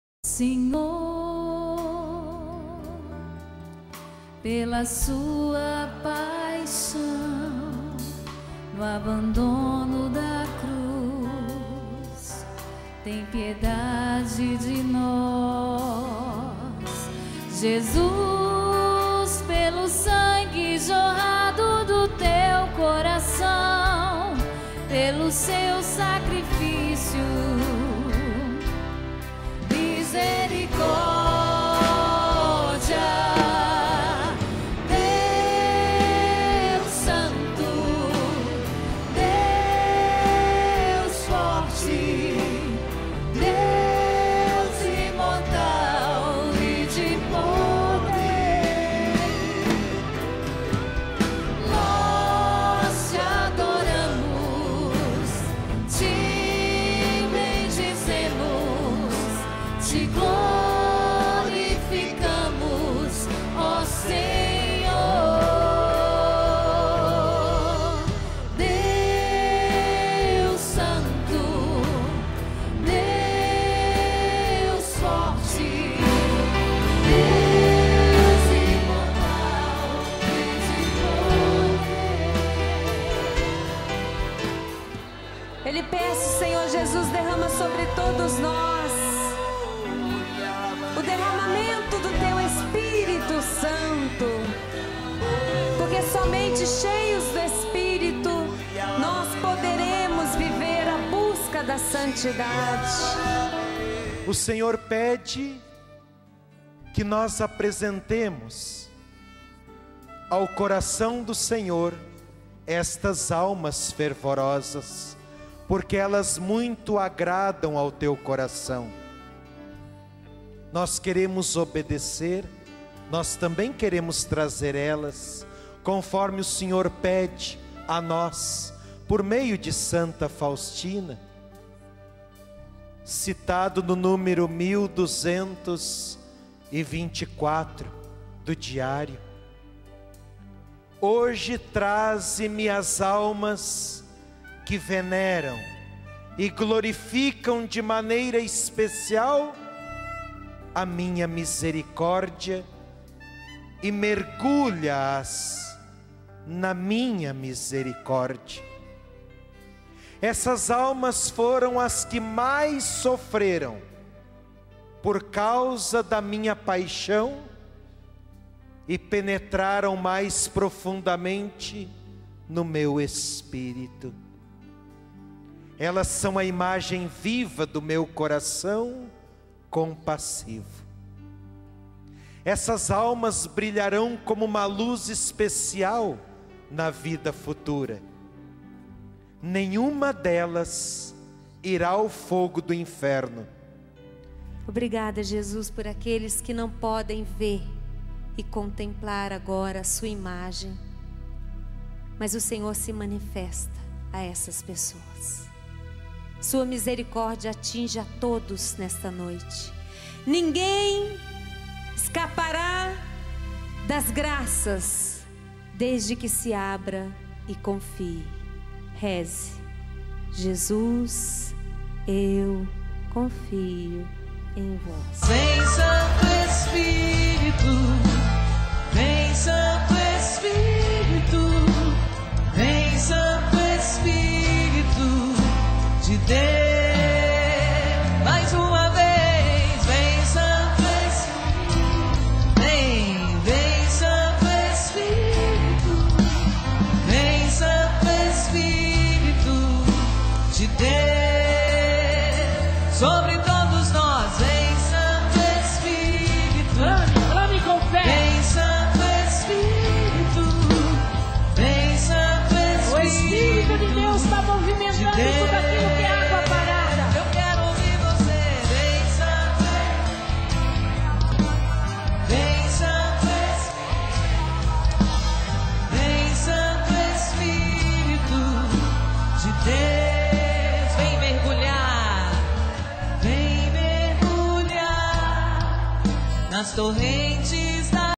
II Encontro da Divina Misericórdia_Jesus eu confio em vós!_
Eu lhe peço Senhor Jesus derrama sobre todos nós o derramamento do teu Espírito Santo,porque somente cheios do Espírito nós poderemos Viver a Busca pela Santidade. As almas fervorosas muito agradam ao Coração de Jesus _

A noite teve início com um momento de animação e logo após, Eliana Sá (Missionária da Canção Nova)rezou com os peregrinos presentes pedindo o batismo no Espírito Santo. A missionária também intercedeu pelas pessoas que acompanhavam em casa pedindo que ouvintes, telespectadores e internautas, enviassem seus pedidos de oração e seus 'impossíveis', para serem depositados aos pés de Jesus misericordioso.
O Grupo de Oração foi marcado por um forte clima de intercessão. Durante a oração, Eliana Sá também suplicou a misericórdia de Deus sobre aqueles que estão a serviço da cultura da morte "recolhe no seu coração misericordioso aquelas pessoas que estão a favor da morte". A missionária também rezou por aquelas crianças que não puderam nascer e também pelas mães que fizeram aborto "quantas almas de crianças Te adoram neste momento Jesus, porque não as deixaram nascer. Pra você que fez um aborto, deixe as suas lágrimas lavarem o seu coração, busque a confissão, Jesus não te condena. Você que cometeu tantos abortos, Jesus te visita nesta hora".

Durante o grupo de oração, as missionárias da Canção Nova Juliana Xavier e Patrícia Mendes também rezaram clamando a misericórdia de Jesus. Orações foram feitas pelos países que estão esfriando na fé, assim como para a Inglaterra, que recebe a visita do Sumo Pontífice, o Papa Bento XVI: "Jesus, eu peço a Tua misericórdia para os sacerdotes, religiosos e missionários que deixaram a vida de intimidade Contigo; onde tudo virou rotina, não há mais a o amor. Reaviva Jesus o ardor e o fervor dos consagrados, para que te busquem" rezou a missionária Patrícia Mendes

Ao som de suplicas e louvores à Divina Misericórdia, Padre Pacheco - diante da imagem de Jesus misericordioso - pediu a todos o fervor espiritual. "O Senhor nos dá a graça do fervor espiritual, nos unindo às almas fervorosas da Divina Misericórdia"

Ao final, Eliana Sá rezou pelas nações, por aqueles que passam por tribulações, pelas pessoas que estão nos hospitais, por aqueles que sofrem com enfermidades e pelos encarcerados.
Padre Pacheco e Eliana Sá.
_18/09/2010

II Encontro da Divina Misericórdia
_ De 17/09/2010 a 19/09/2010
Canção Nova_ Cachoeira Paulista